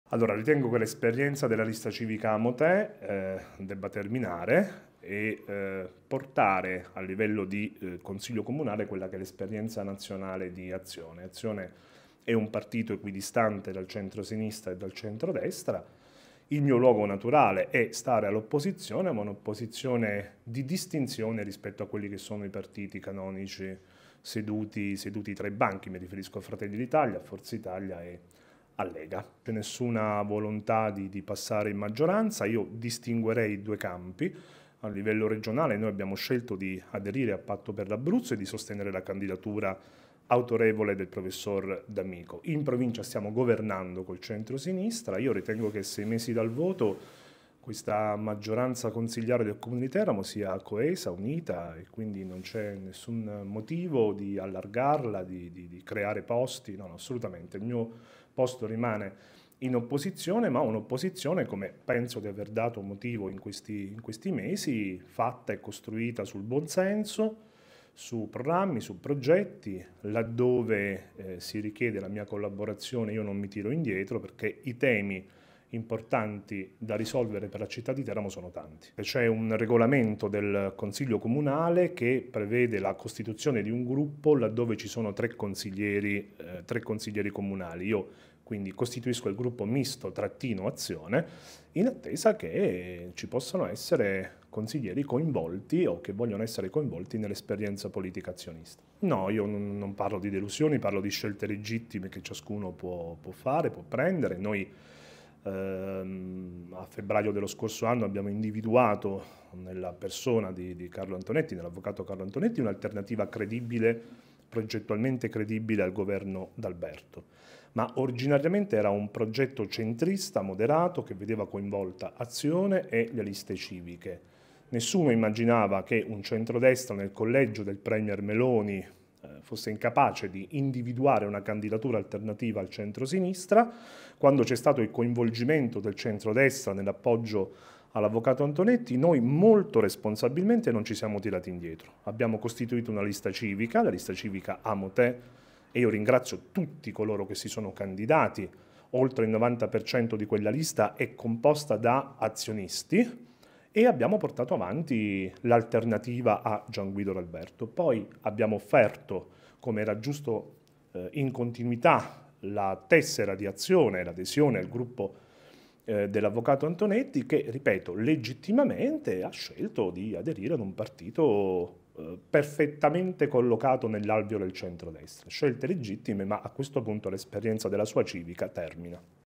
A Teramo Azione va nel gruppo misto ma resta in minoranza
La decisione di Alessio D'Egidio, consigliere e segretario provinciale di Azione